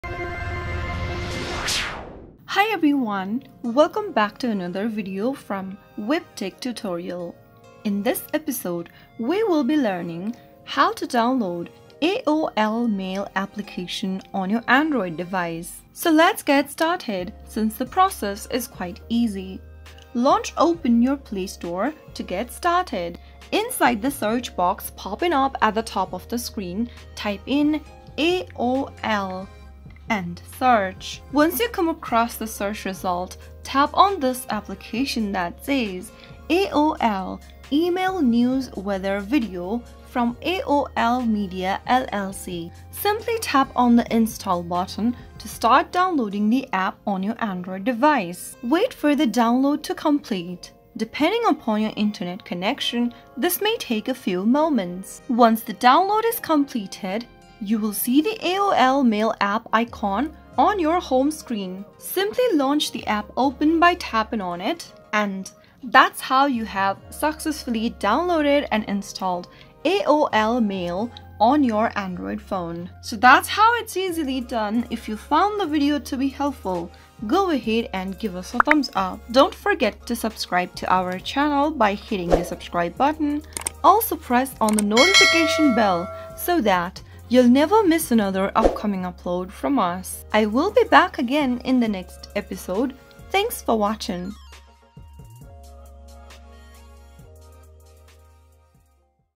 How To Download AOL Mail App On Android 2024?
Discover the straightforward process of downloading the AOL Mail app on your Android device with our comprehensive guide. Follow our step-by-step instructions to ensure a smooth installation. Don't miss out on the convenience of having AOL Mail at your fingertips. Watch now and simplify your email management on Android!